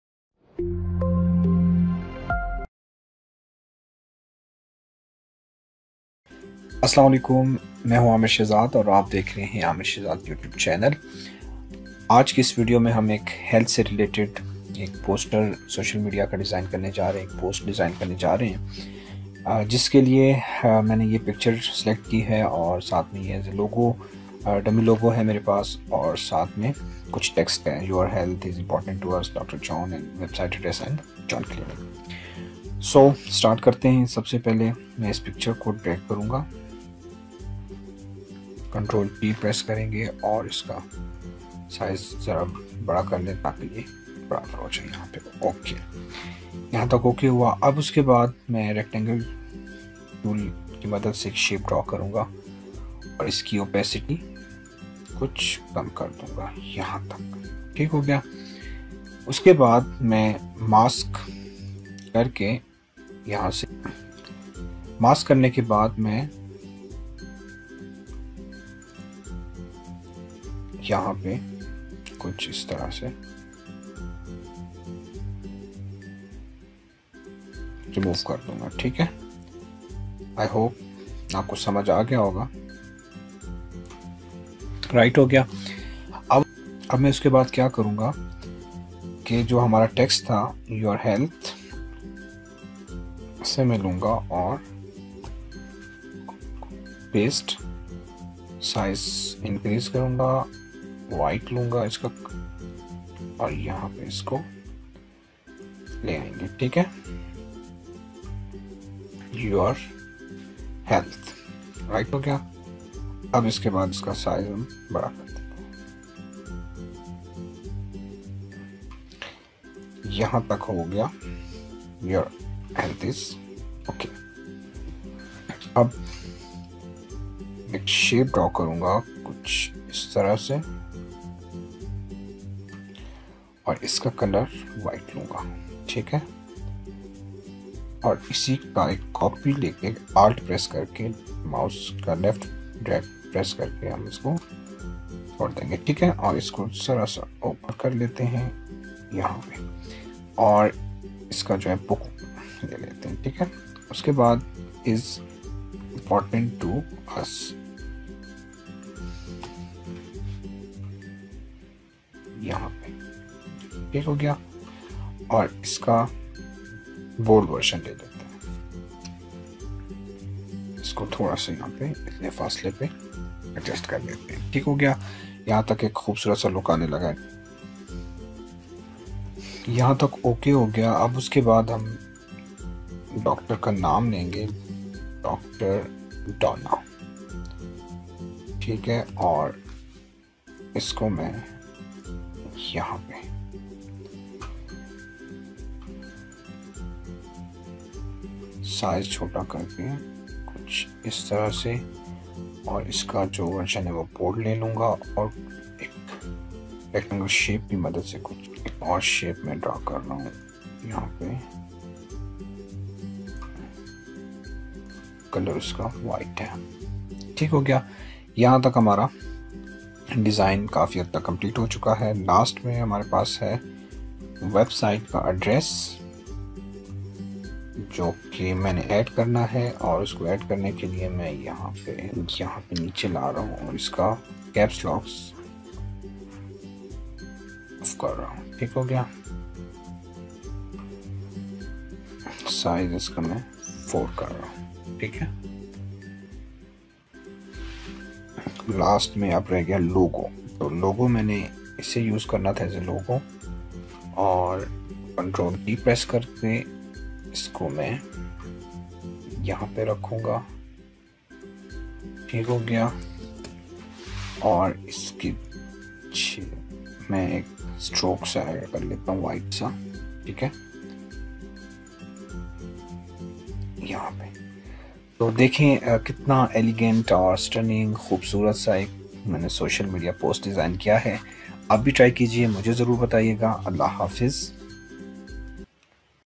Health | Social Media Post for Graphic design | Photoshop CC| Tutorial
In this Video, You can learn how i designed health social media post design by using photoshop cc 2020.
If you want to design Doctor social media post design in photoshop, it is the best tutorial for you.